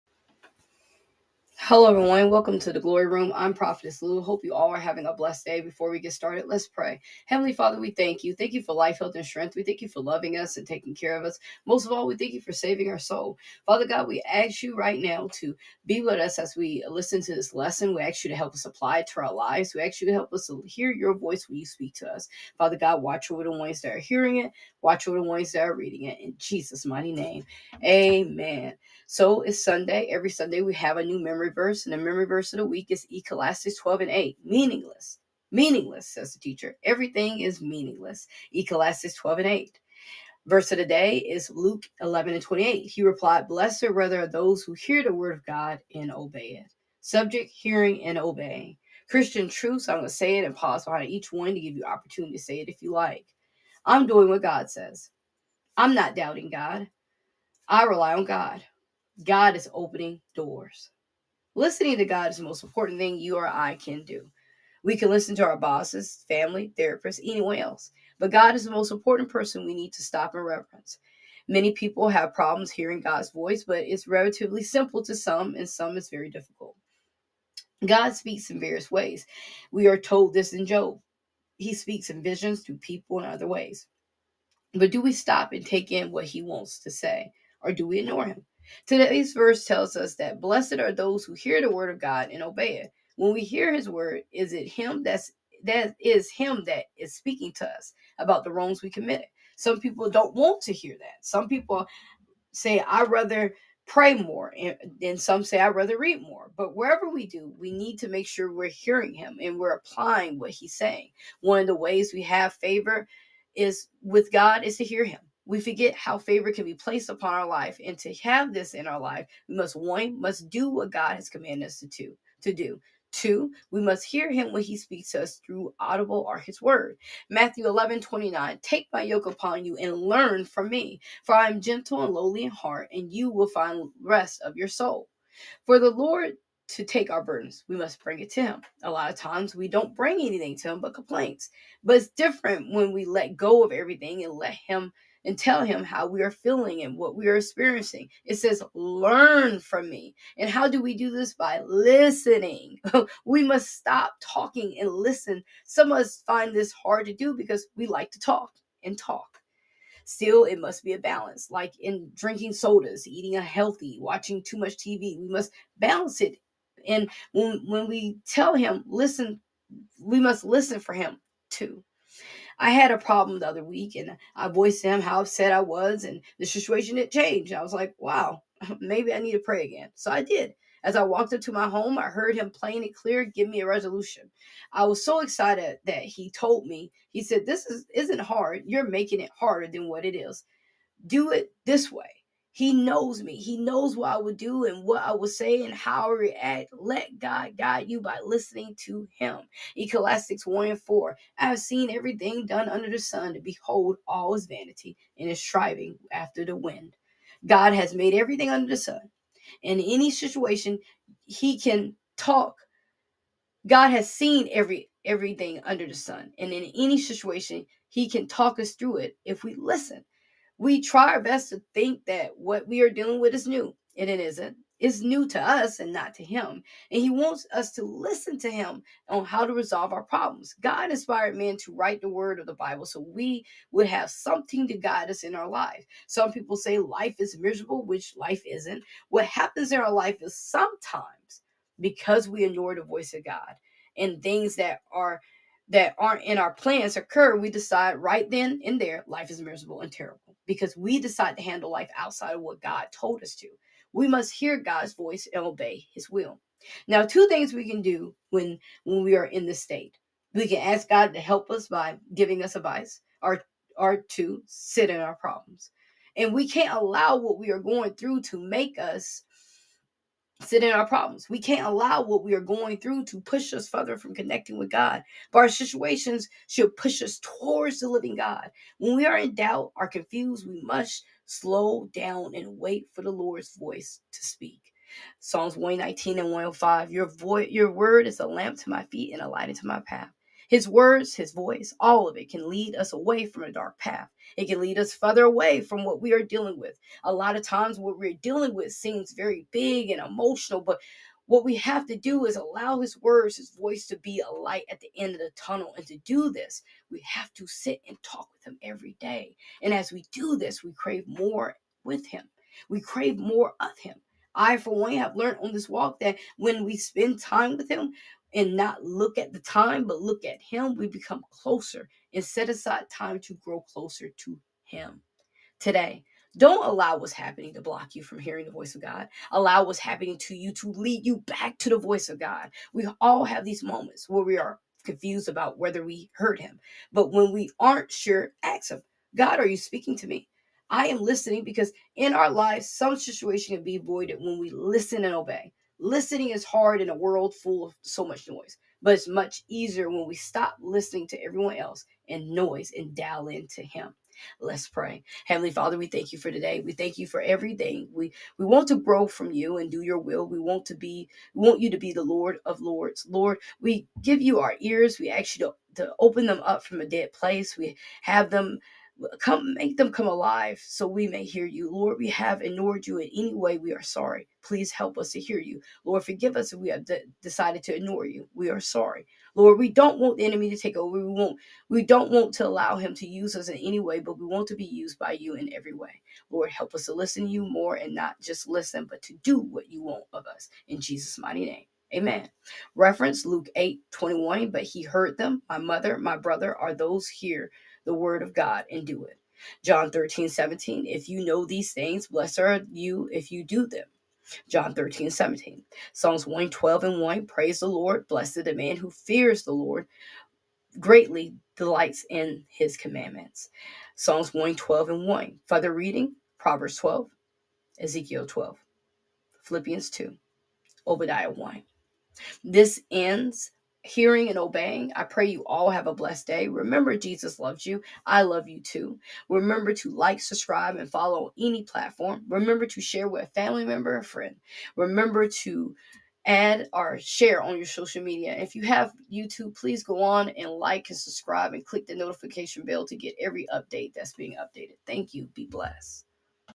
Hearing & Obeying
MEMORY VERSE OF THE WEEK: + Ecclesiastes 12:8 Meaningless! Meaningless!” says the Teacher. “Everything is meaningless!”

VERSE OF THE DAY: + Luke 11:28 He replied, “Blessed rather are those who hear the word of God and obey it.”
REFERENCES
+ Luke 8:21 But he answered them, “My mother and my brothers are those who hear the word of God and do it.”
+ John 13:17 If you know these things, blessed are you if you do them.
+ Psalm 112:1 Praise the Lord! Blessed is the man who fears the Lord, greatly delights in his commandments!

FURTHER READINGS
PROVERBS 12
EZEKIEL 12
PHILIPPIANS 2
OBADIAH 1